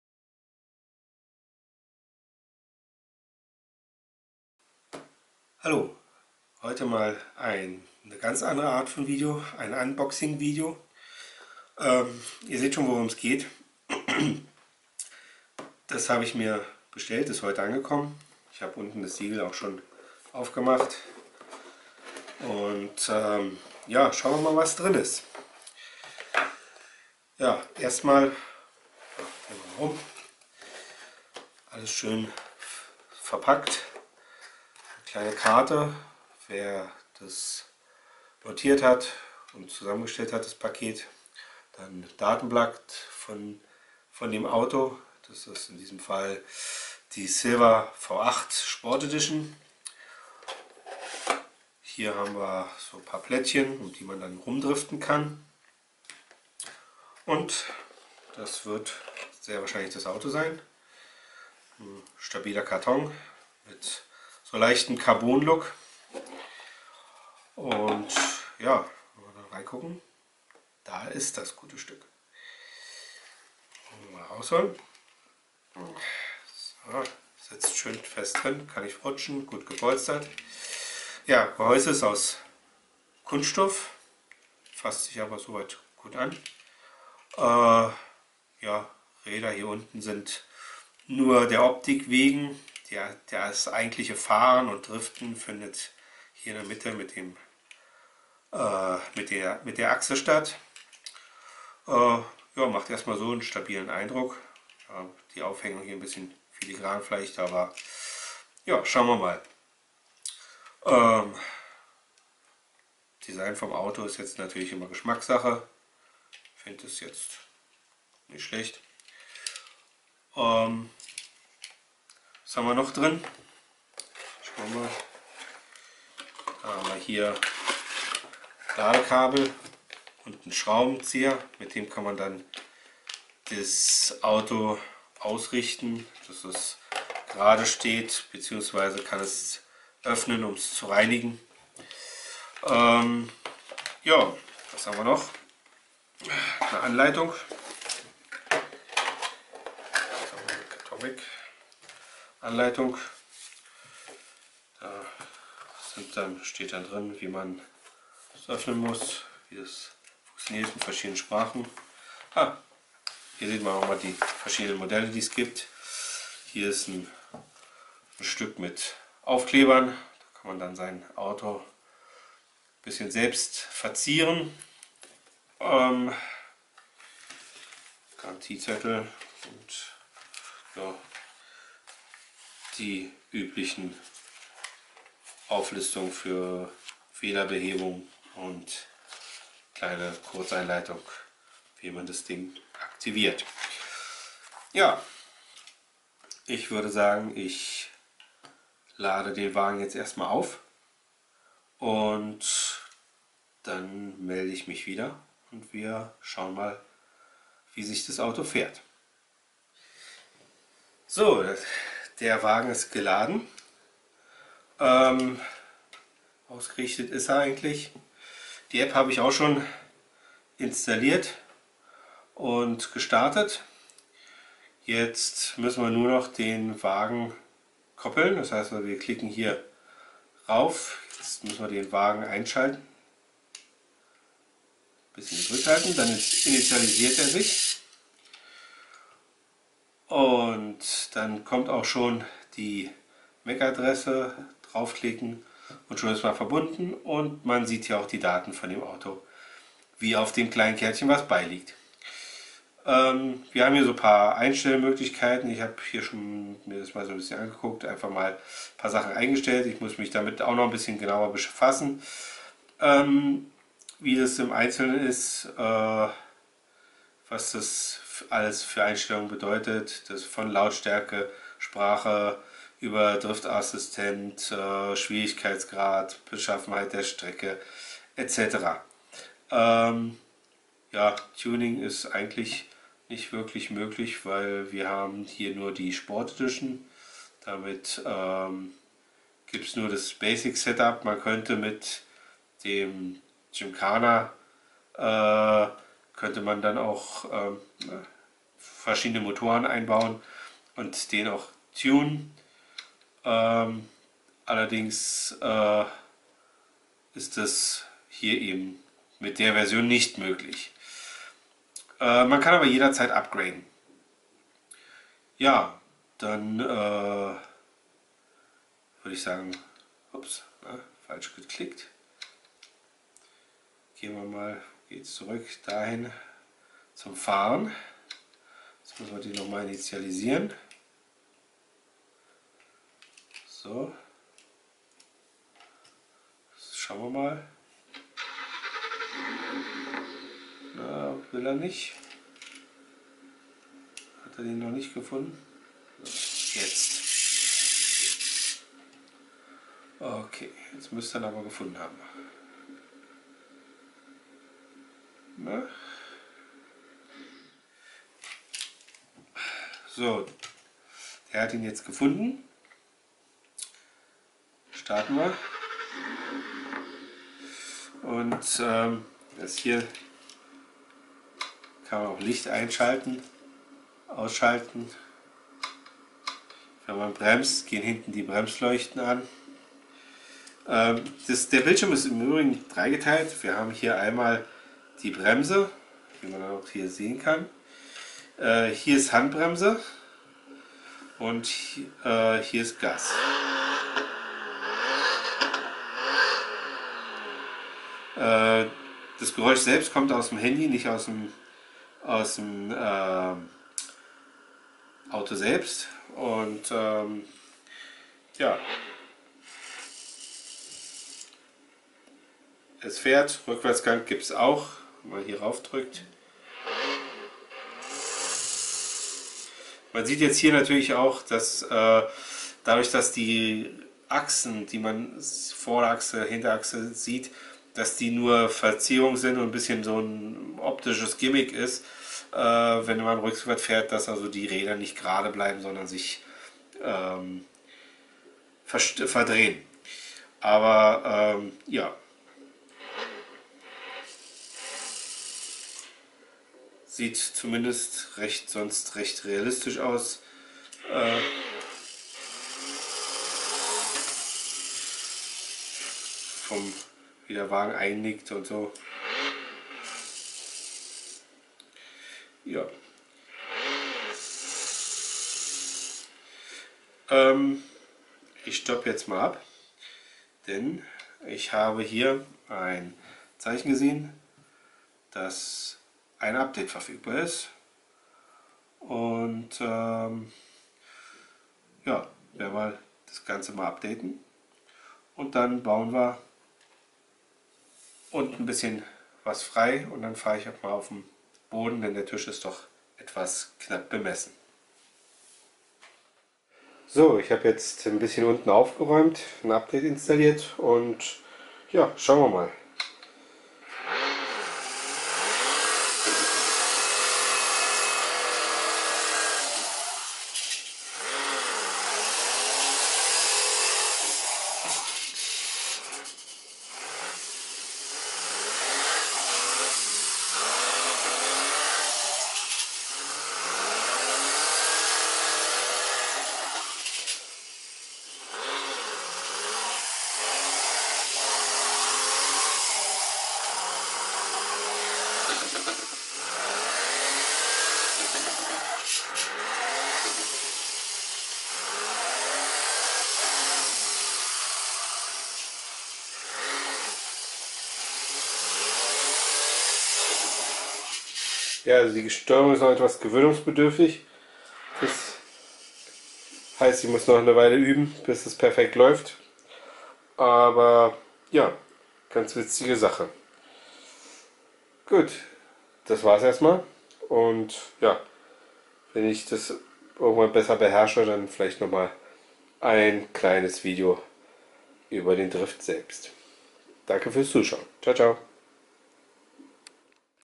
Unboxing: DR!FT
Ich hab mit eines dieser kleinen Dr!ft-Autos gekauft und stelle es euch hier mal vor.